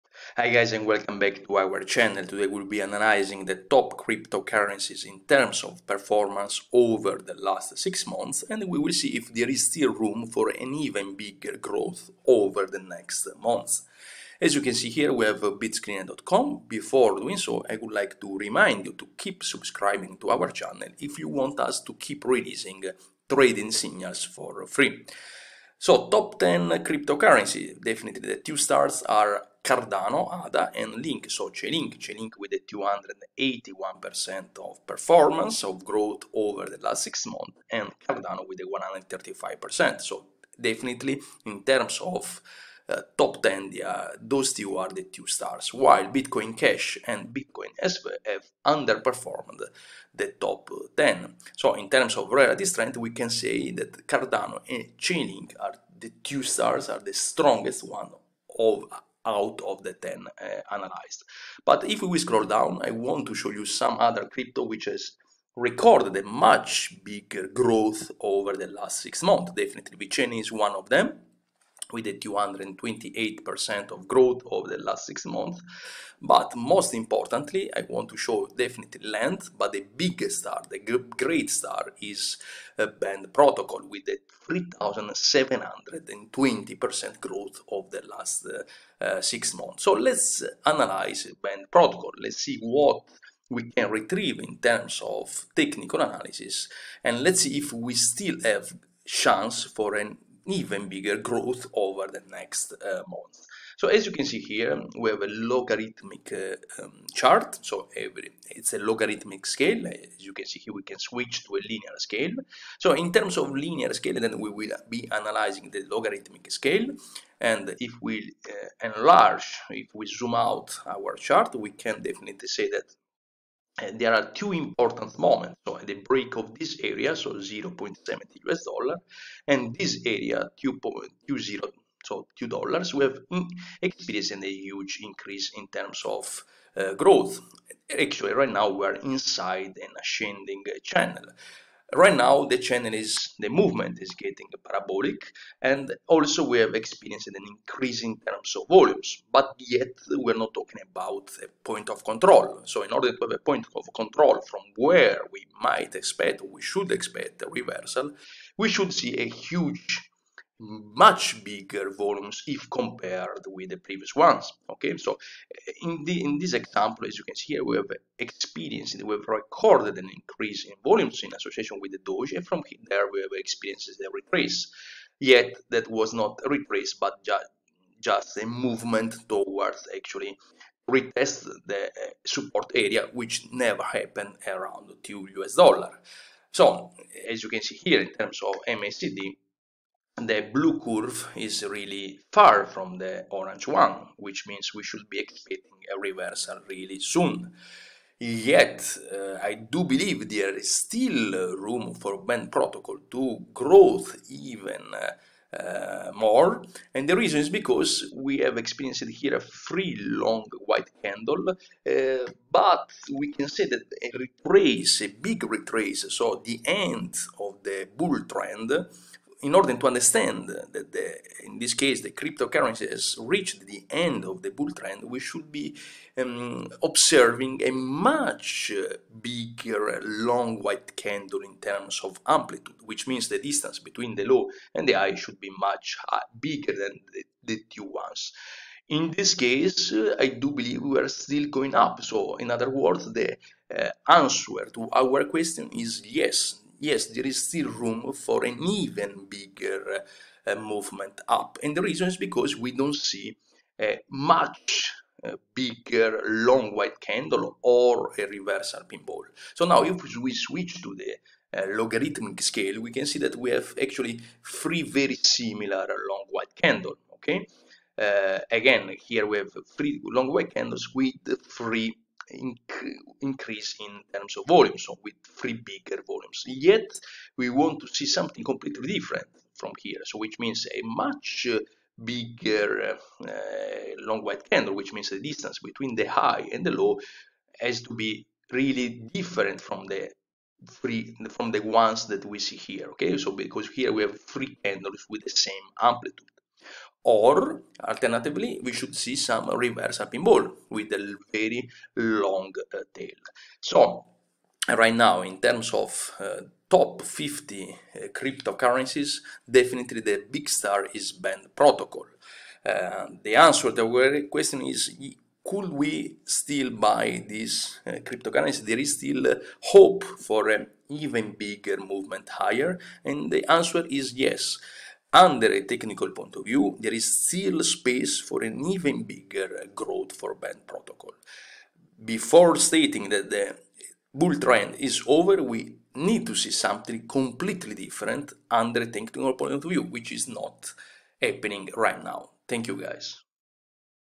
Band Protocol (BAND), Cardano (ADA), Vetchain (VET) technical analysis. Will BAND keep going up?
Band Protocol (BAND), Cardano (ADA), Vetchain (VET) technical analysis.
Will BAND keep going up?

- Multi-awarded best selling forex book: "Forex the Holy Grail"
- Fb page: Financial Trading Academy @newtradingacademy